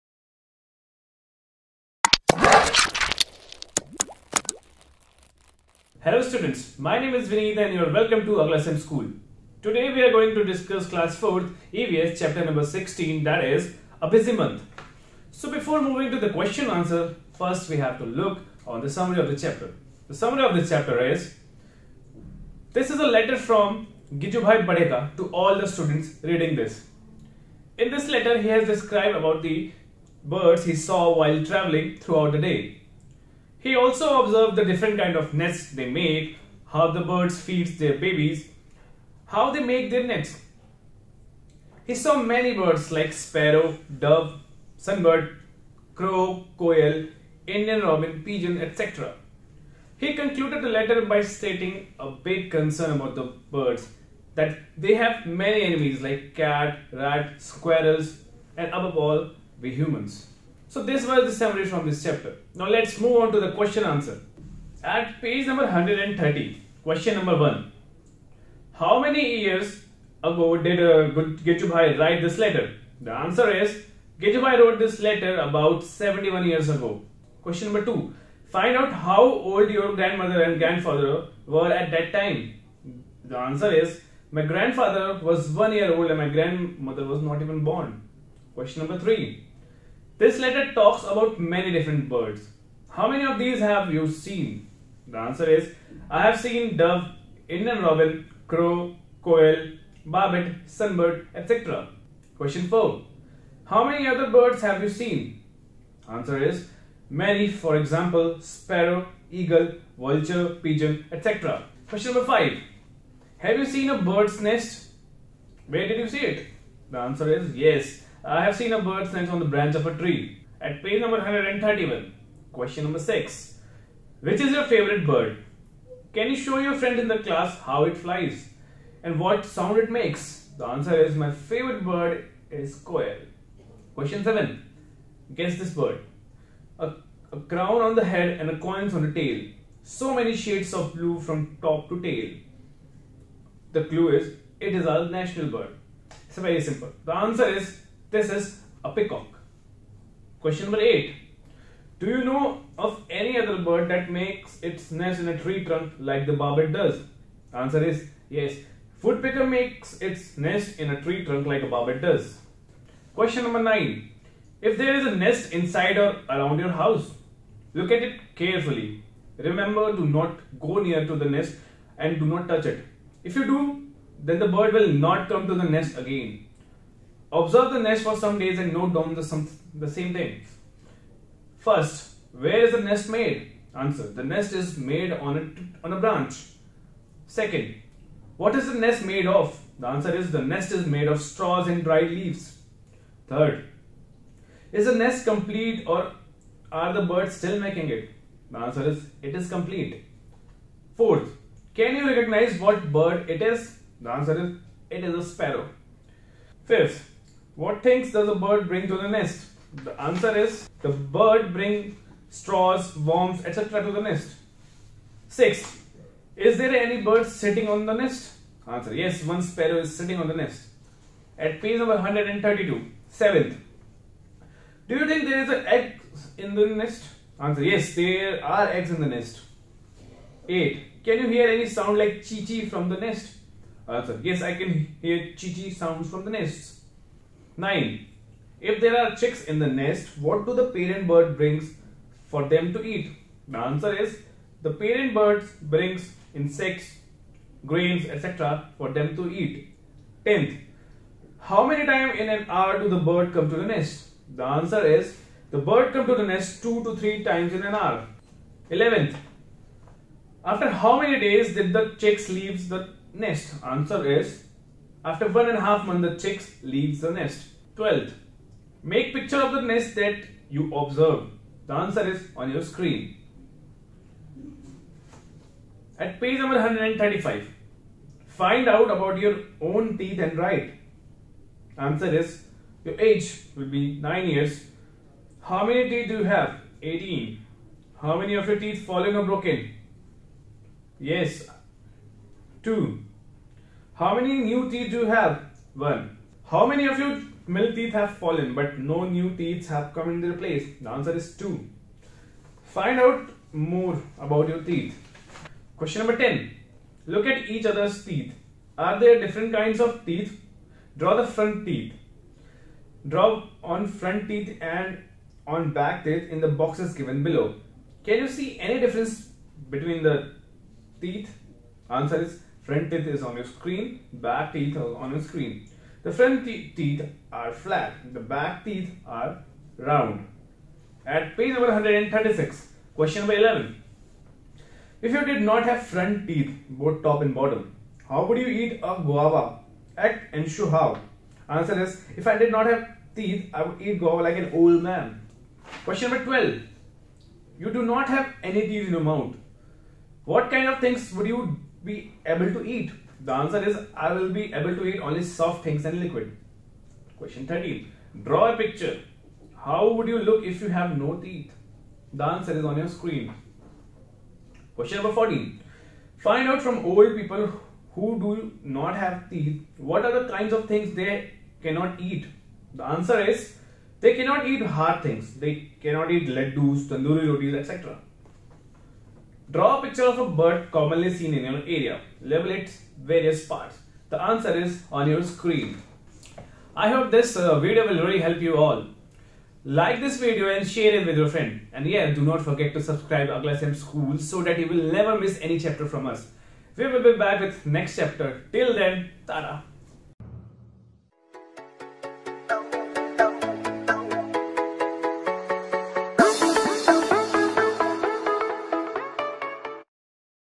NCERT Solutions Class 4 EVS Chapter 16 A Busy Month
4th Class EVS NCERT Solutions Chapter 16 - A Busy Month

This video includes the detailed solutions of Class 04 Subject EVS Textbook Chapter Number 16 titled A Busy Month. It includes solutions to all the exercises given the NCERT textbook prescribed by CBSE. The questions answers for the class 04 Subject NCERT textbook have been provided in order to help the students understand the subject better.

Summary: This is a letter from Gijubhai Badheka to all the children reading this. in this letter, he has described the birds he saw while traveling throughout the day. He also observed the different kinds of nests they make, how the birds feed their babies, how they make their nests. he saw many birds like a Sparrow, Dove, Sunbird, Crow, Koel, Indian Robin, Pigeons etc. he concluded the letter by stating a very big concern about the birds that they have many enemies like a cat, rat, squirrels and above all we humans.

For other chapters direct links are given below.